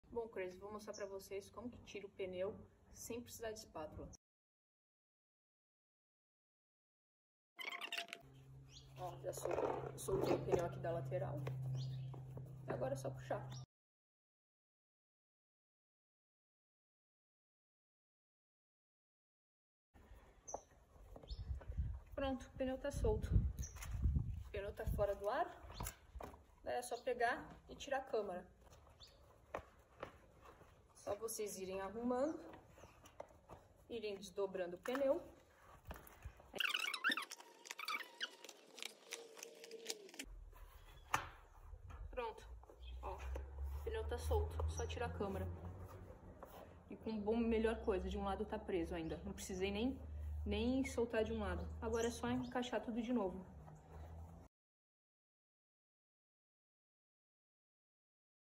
Como tirar o pneu sem precisar da espátula!
Quem vai para a trilha sem espátula?

Pois é, eu vou!!!

Mas calma, eu consigo tirar o pneu do aro sem precisar da espátula.

Sim, os pneus de hoje em dia estão mais fáceis de tirar sem a necessidade de uma espátula.

Eu só recomendo para pneu de mtb pneu de speed eu ainda recomendo usar espátula pra não ficar sem unha, mas dá para tirar também!

Me segue no Instagram: @loucadoendurance